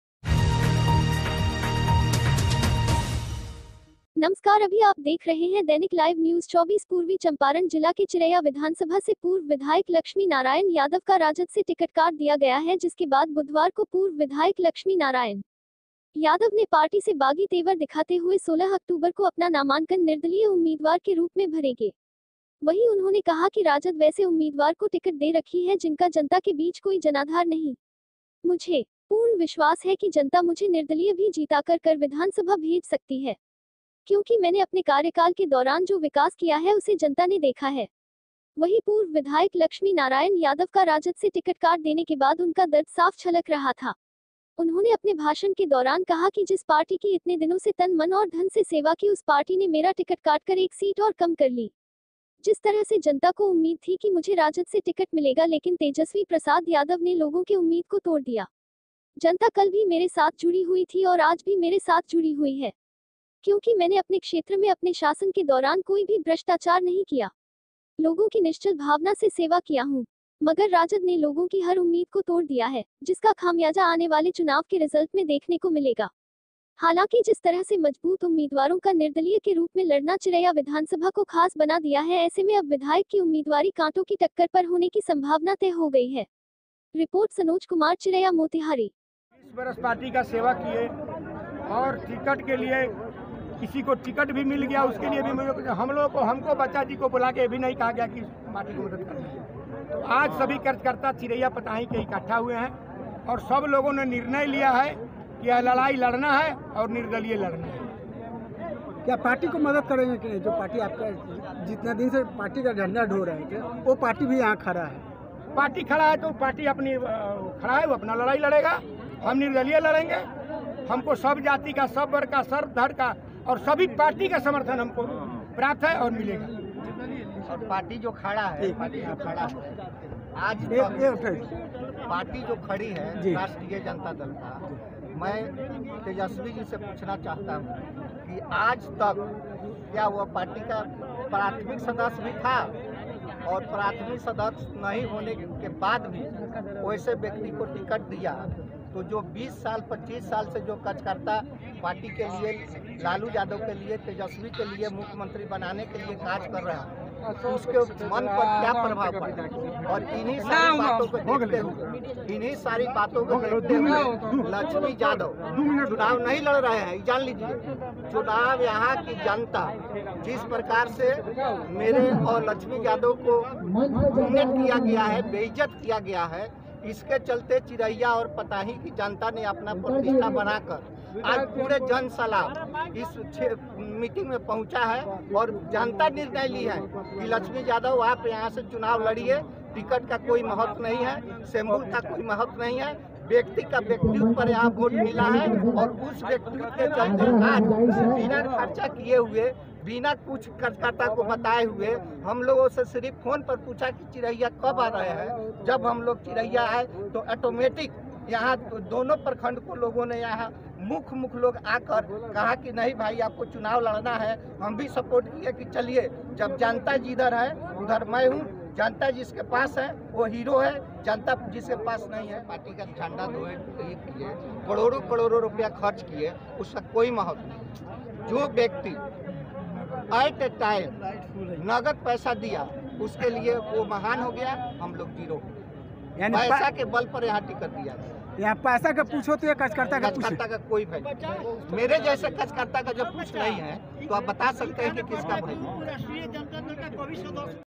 पूर्व विधायक लक्ष्मी नारायण यादव निर्दलीय चुनाव लड़ने की घोषणा
चिरैया विधानसभा से पूर्व विधायक लक्ष्मी नारायण यादव का राजद ने टिकट काट दिया गया है जिसके बाद आज पूर्व विधायक लक्ष्मी नारायण यादव ने पार्टी से बागी तेवर दिखाते हुए 16 अक्टूबर को अपना नामांकन निर्दलीय उम्मीदवार के रूप में भरेंगे। वहीं उन्होंने कहा कि राजद वैसे उम्मीदवार को टिकट दे रखी है जिनका जनता के बीच कोई जनाधार नहीं। मुझे पूर्ण विश्वास है की जनता मुझे निर्दलीय भी जीताकर कर विधानसभा भेज सकती है, क्योंकि मैंने अपने कार्यकाल के दौरान जो विकास किया है उसे जनता ने देखा हैवही पूर्व विधायक लक्ष्मी नारायण यादव का राजद से टिकट काट देने के बाद उनका दर्द साफ छलक रहा था। उन्होंने अपने भाषण के दौरान कहा की जिस पार्टी की इतने दिनों से तन मन और धन से सेवा की उस पार्टी ने मेरा टिकट काटकर एक सीट और कम कर ली। जिस तरह से जनता को उम्मीद थी कि मुझे राजद से टिकट मिलेगा लेकिन तेजस्वी प्रसाद यादव ने लोगों के उम्मीद को तोड़ दिया। जनता कल भी मेरे साथ जुड़ी हुई थी और आज भी मेरे साथ जुड़ी हुई है, क्योंकि मैंने अपने क्षेत्र में अपने शासन के दौरान कोई भी भ्रष्टाचार नहीं किया। लोगों की निश्चल भावना से सेवा किया हूं। मगर राजद ने लोगों की हर उम्मीद को तोड़ दिया है, जिसका खामियाजा आने वाले चुनाव के रिजल्ट में देखने को मिलेगा।
हालांकि जिस तरह से मजबूत उम्मीदवारों का निर्दलीय के रूप में लड़ना चिरैया विधानसभा को खास बना दिया है ऐसे में अब विधायक की उम्मीदवारी कांटो की टक्कर पर होने की संभावना तय हो गई है।